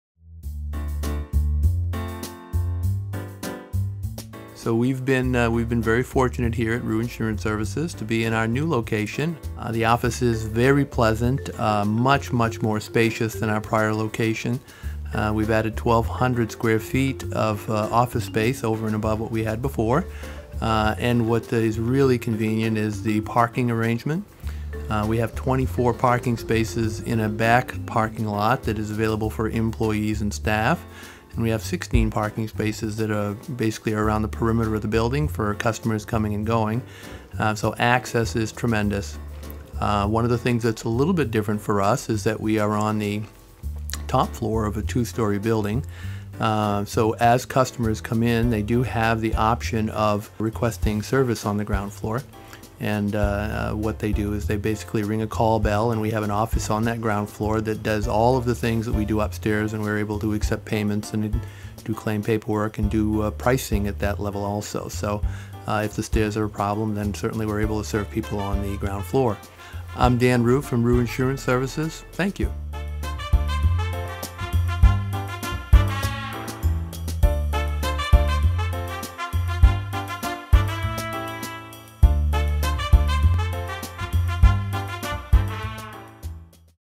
Roux Insurance Services - New Office
Come by and see Roux Insurance's new location at 185 Webster Street in Lewiston.